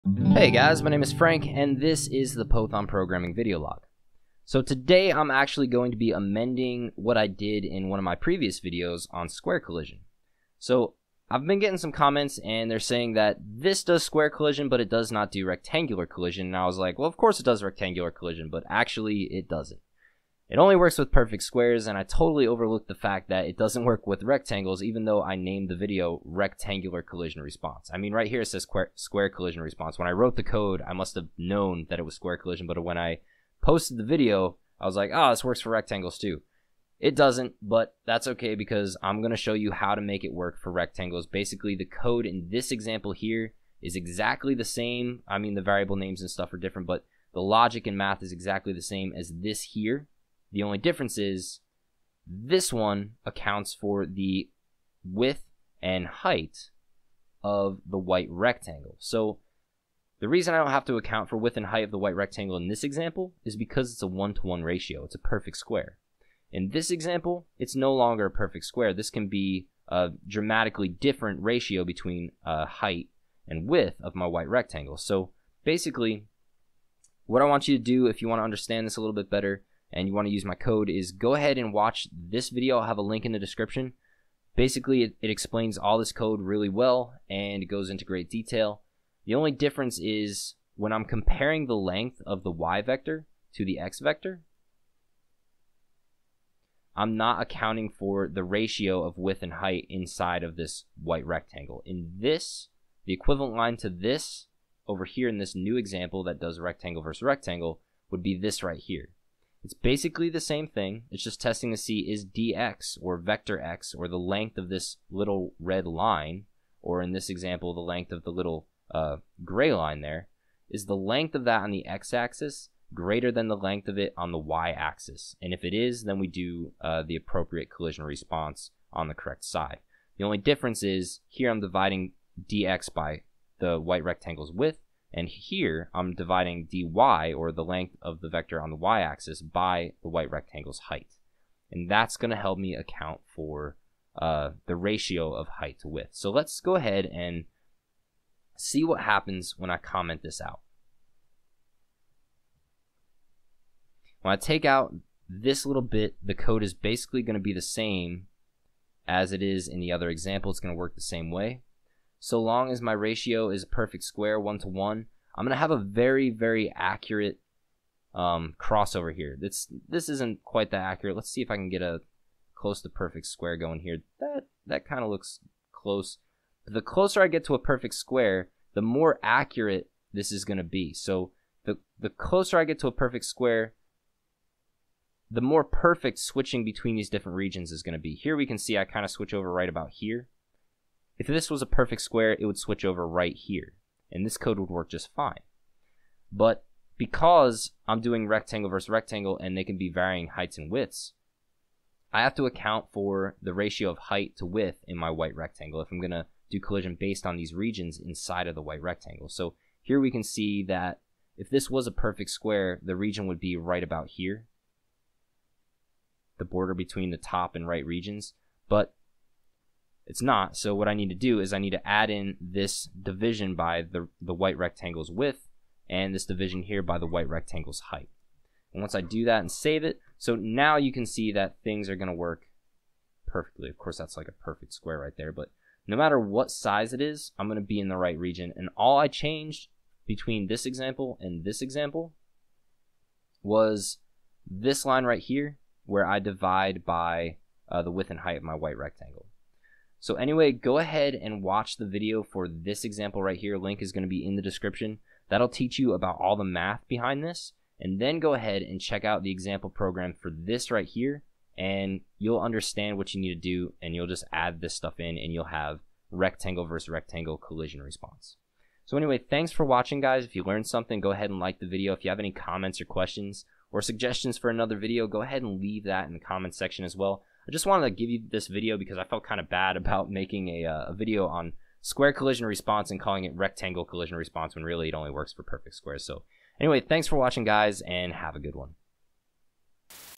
Rectangular Collision Detection And Response In JavaScript
Learn how to do Rectangular Collision Response using pure JavaScript in this short video tutorial that amends my video on Square Collision response.